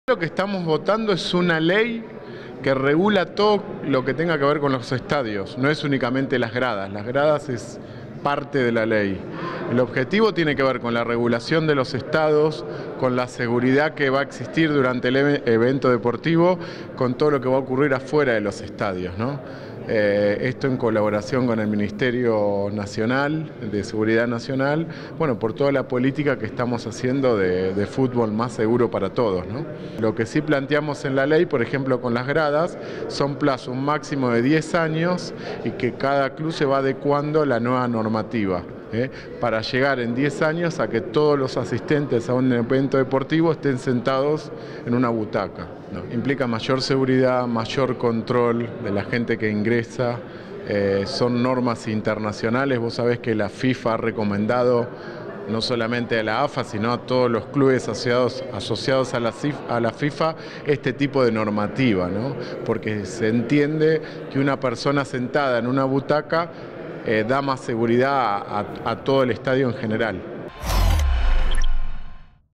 Debaten una ley para regular las actividades deportivas en los estadios
La Legislatura Porteña tiene en tratamiento una nueva normativa que regula las actividades deportivas en los estadios de fútbol de la Ciudad de Buenos Aires. La iniciativa establece que en los próximos diez años los clubes adapten sus tribunas para que la totalidad de los espectadores ocupen asientos y se finalice con las tribunas populares.